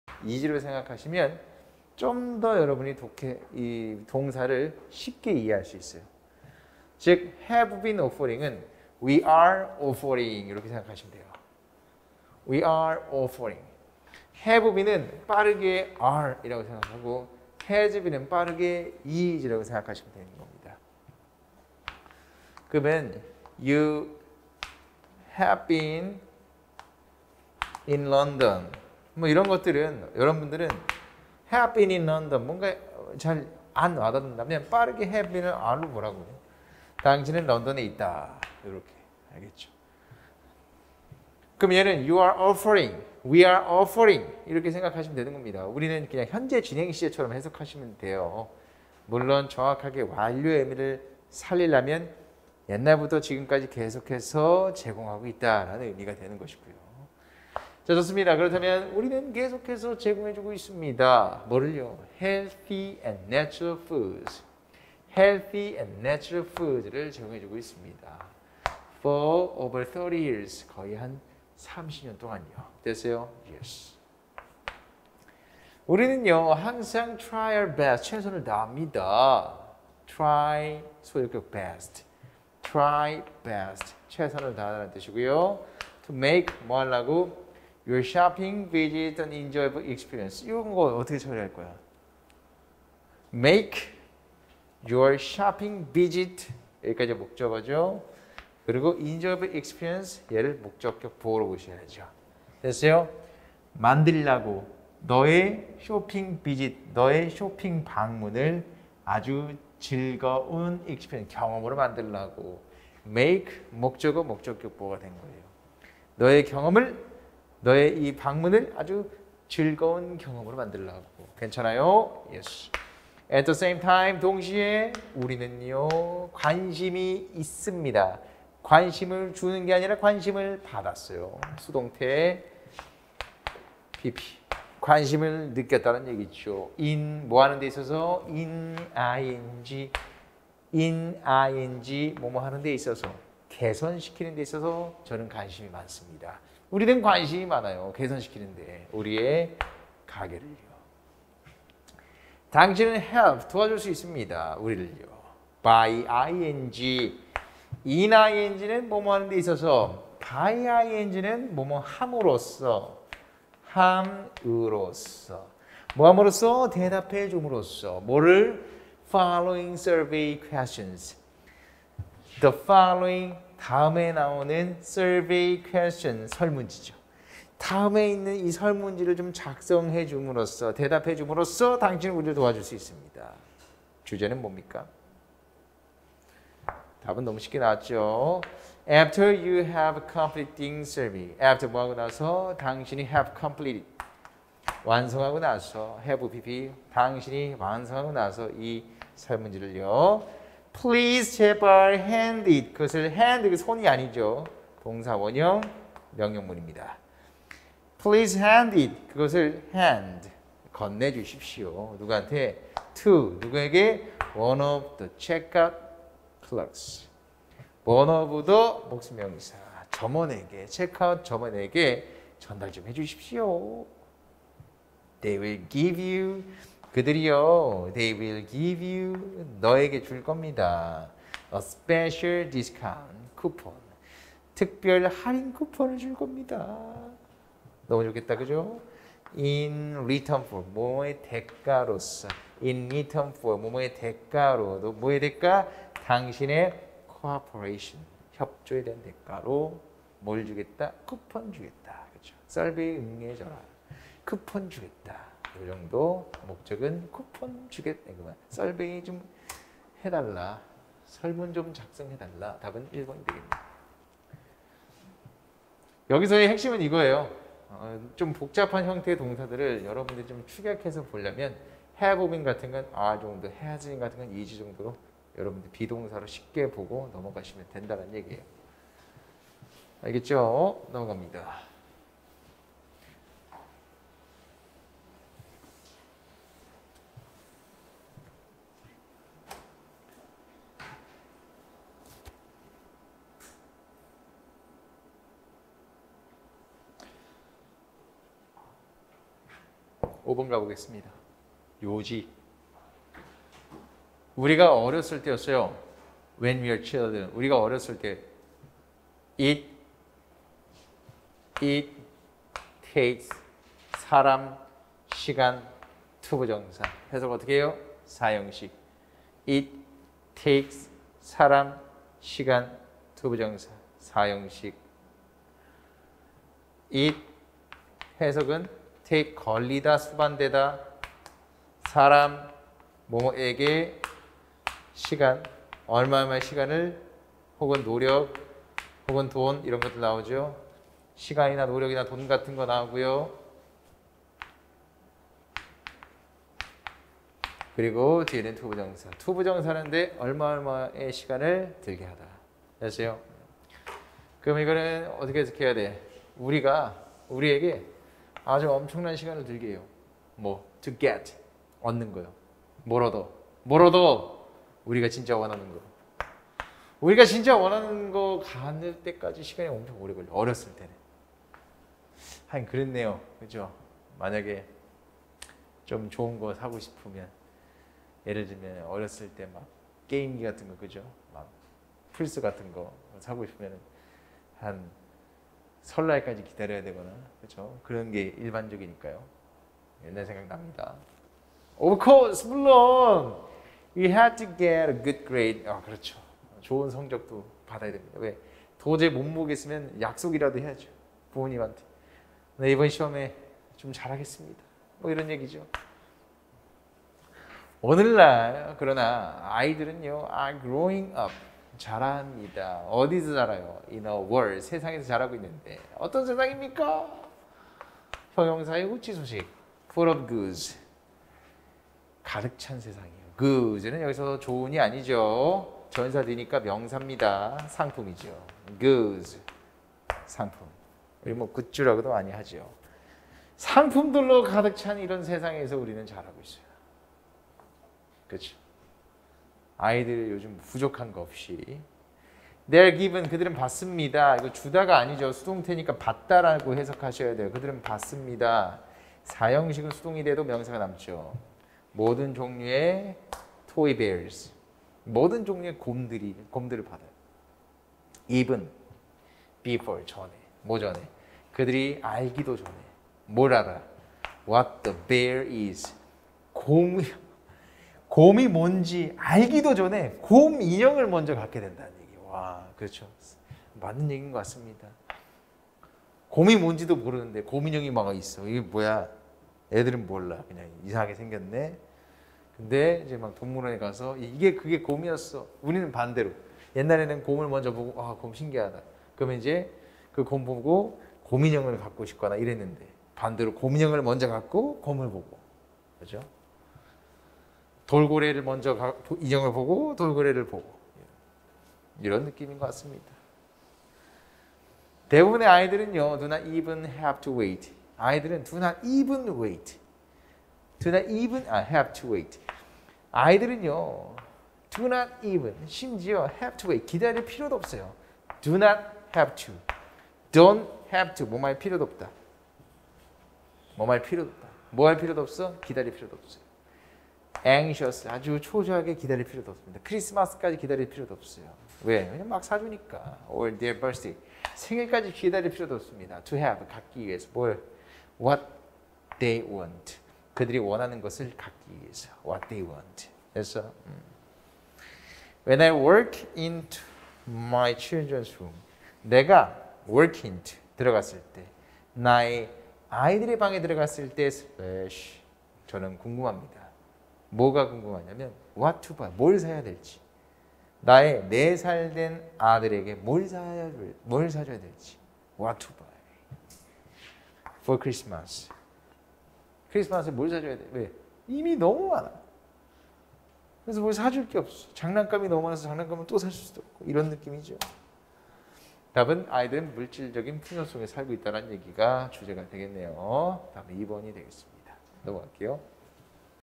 윤광덕공무원영어_have been? has been? 어떻게 처리하지?독해팁! 이렇게 해보자
지문이 긴 독해에서 have been과 has been은 어떻게 처리할까요?
보다 빠르게 풀 수 있는 독해팁을 알아보시죠

▶지안에듀 상담하기: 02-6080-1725